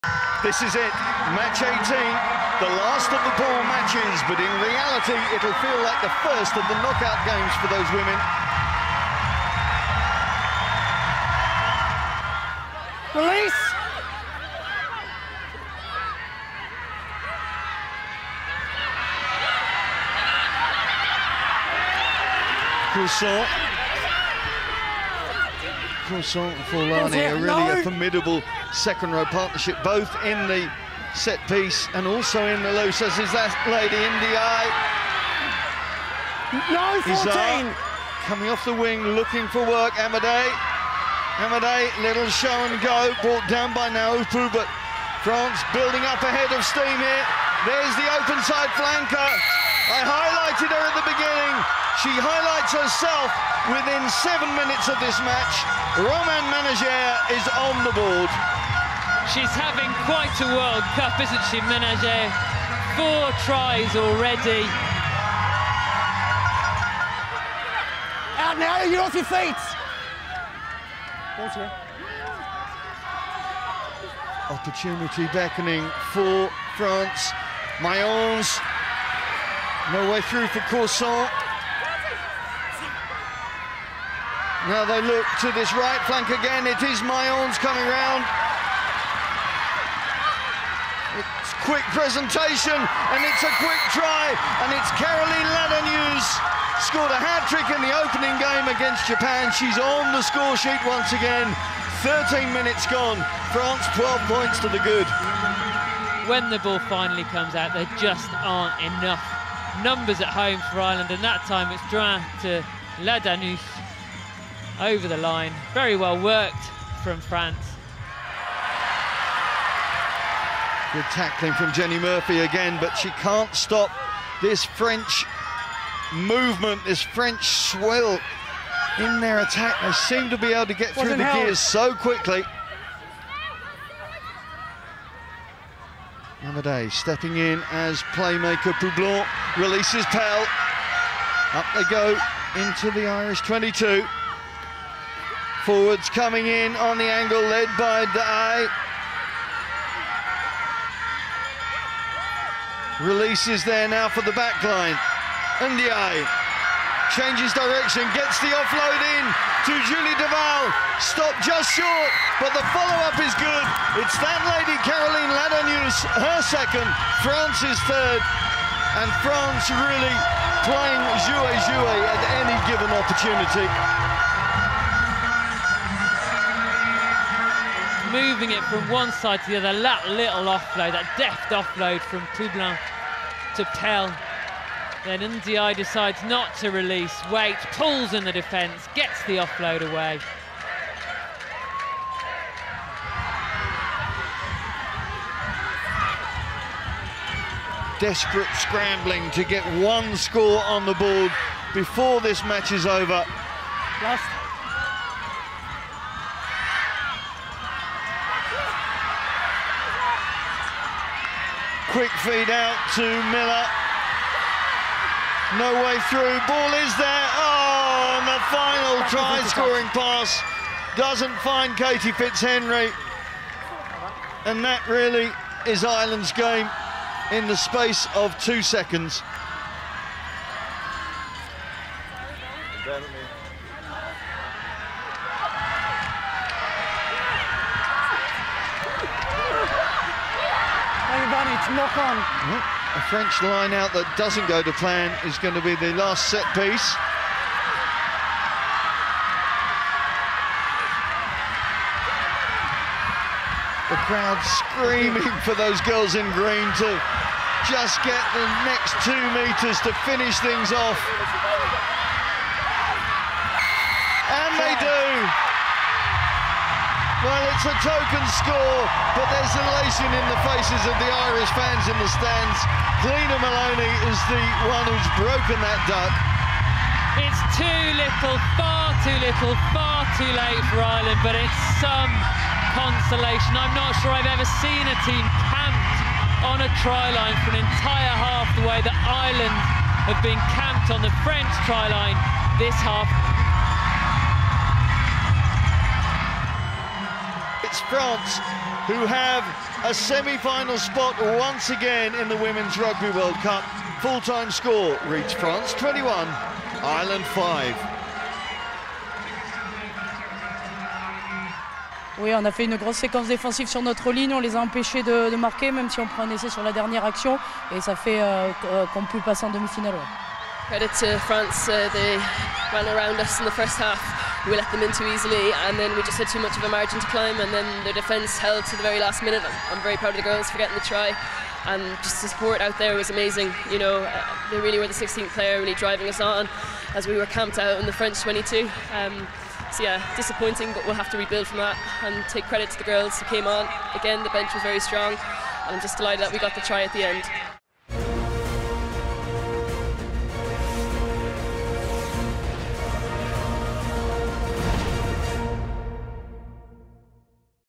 WRWC Highlights: France beat Ireland 21 - 5 to progress to the semi-finals
Another thrilling game at UCD where France proved to strong for Ireland at the Women's Rugby World Cup